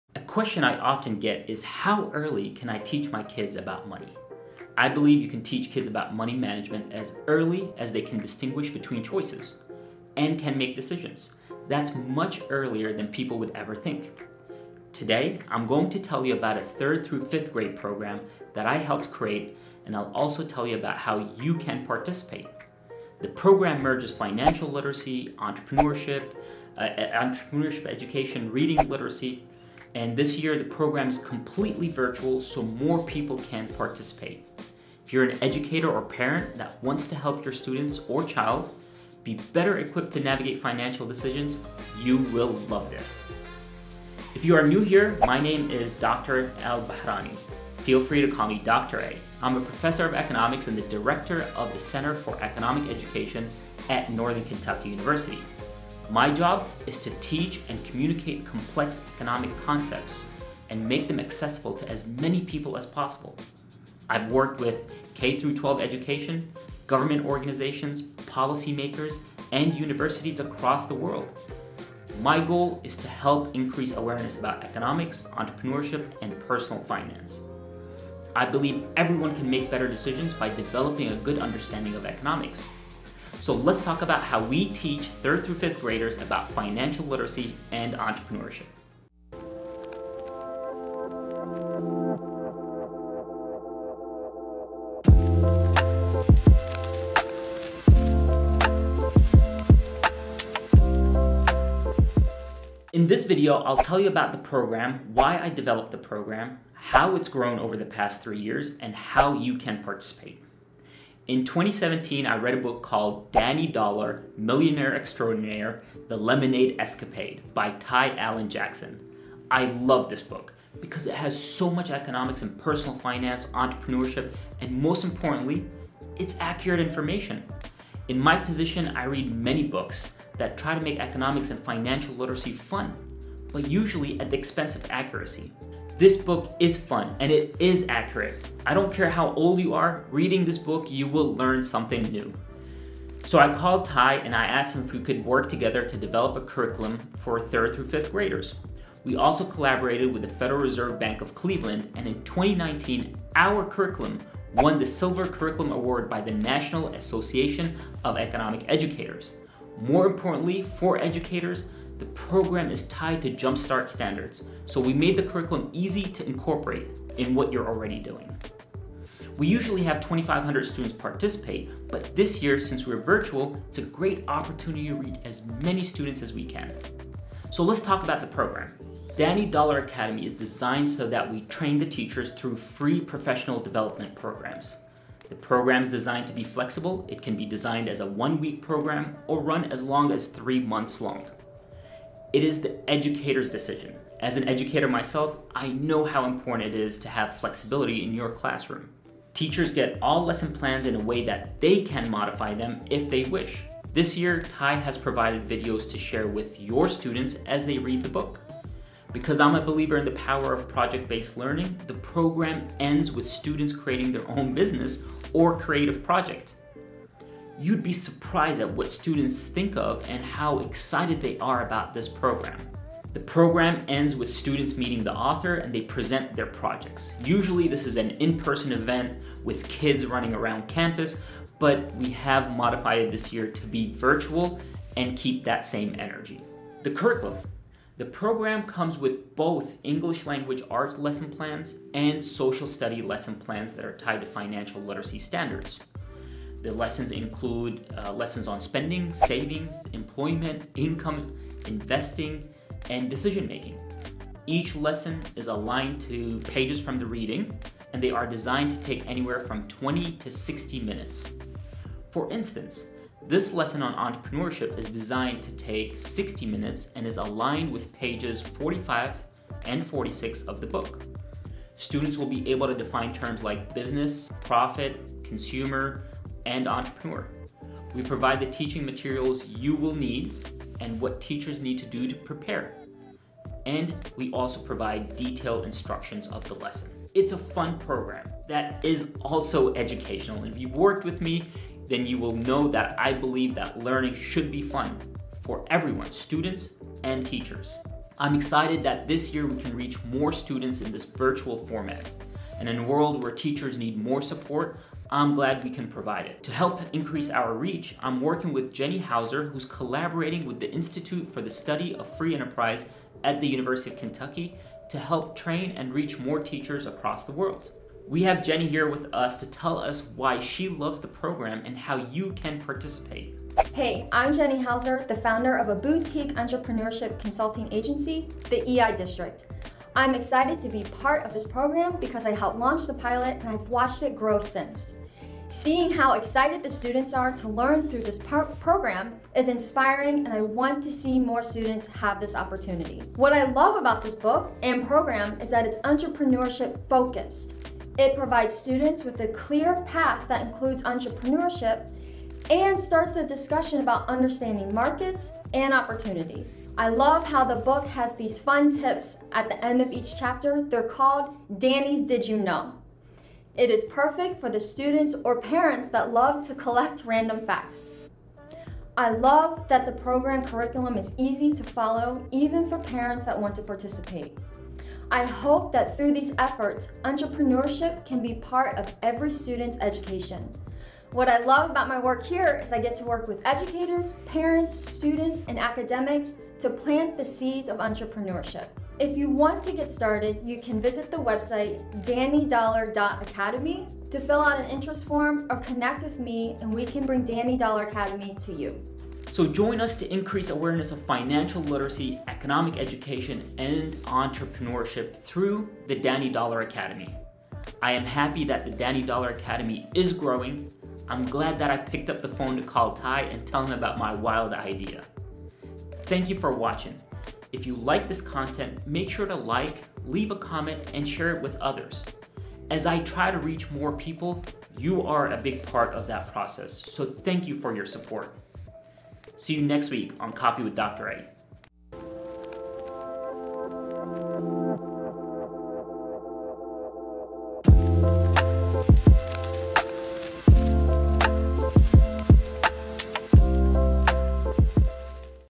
Danny Dollar Academy.
Recommended Videos
Jobs for economics I often get asked, what's the best way to teach kids about money? In this episode of CoffeeWithDrA, I tell you how!